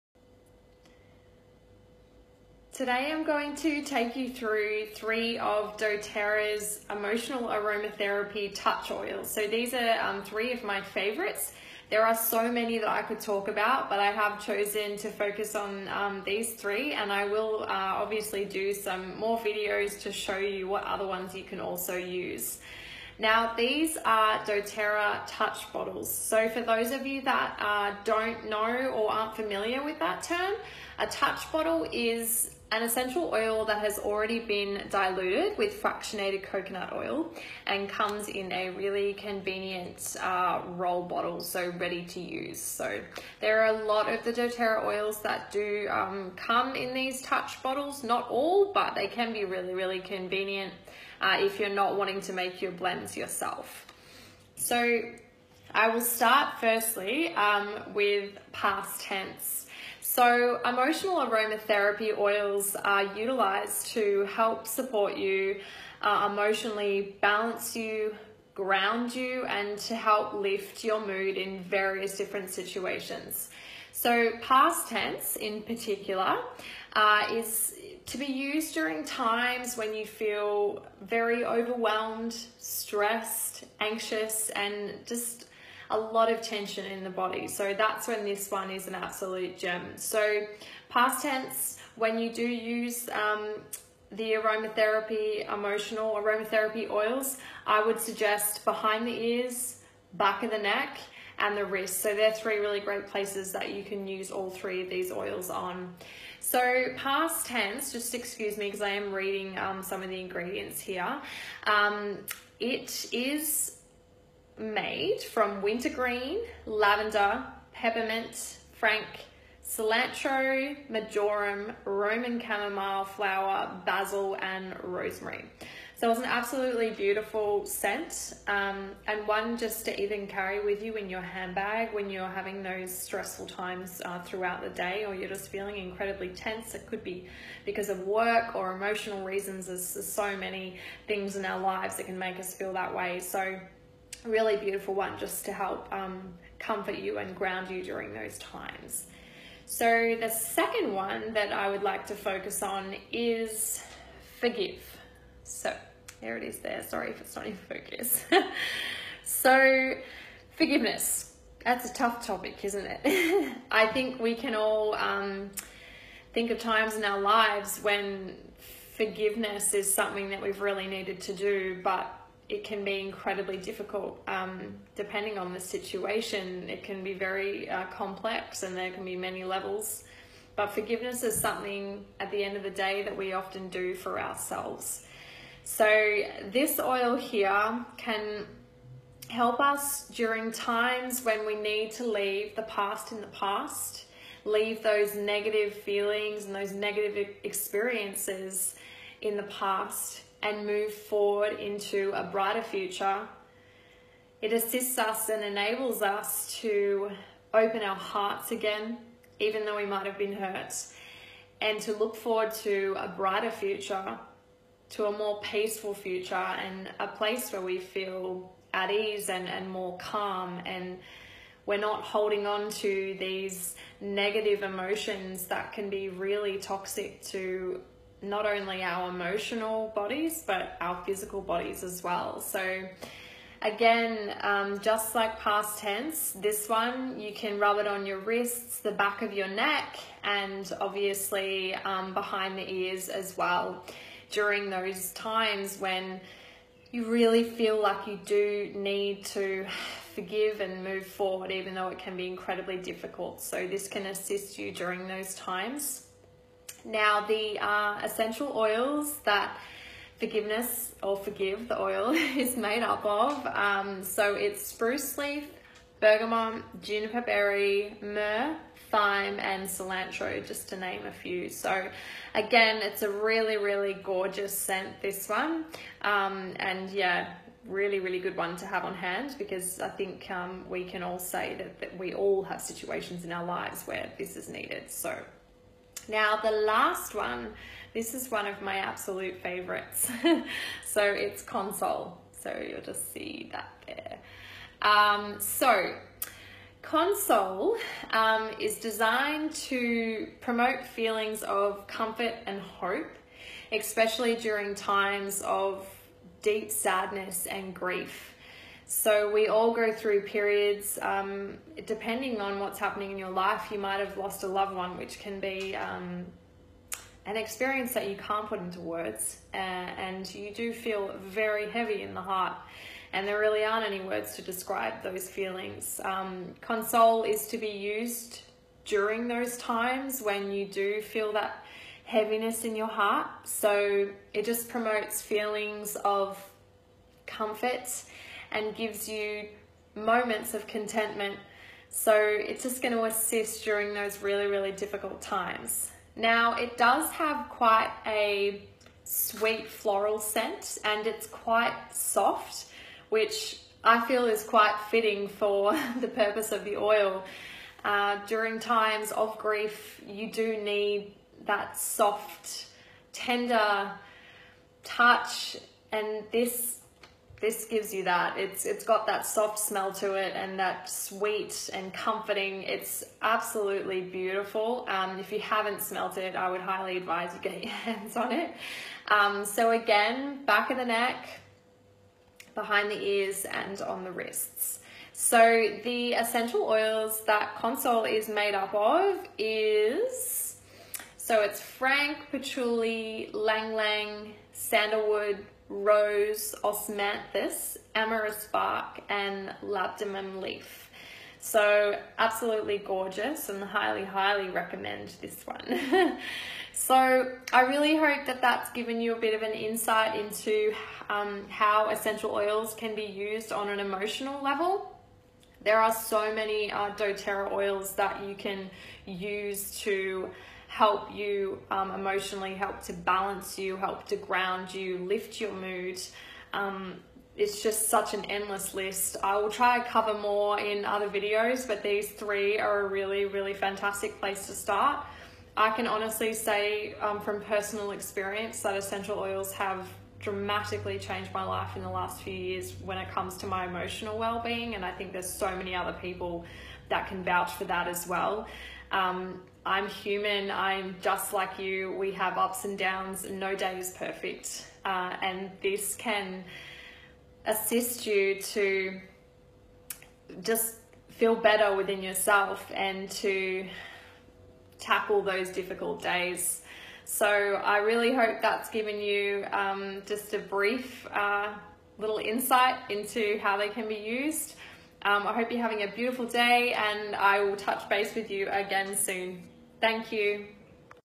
Emotional Aromatherapy
Emotional aromatherapy: doTERRA essential oils that help provide emotional support during difficult times

Light & love

B xx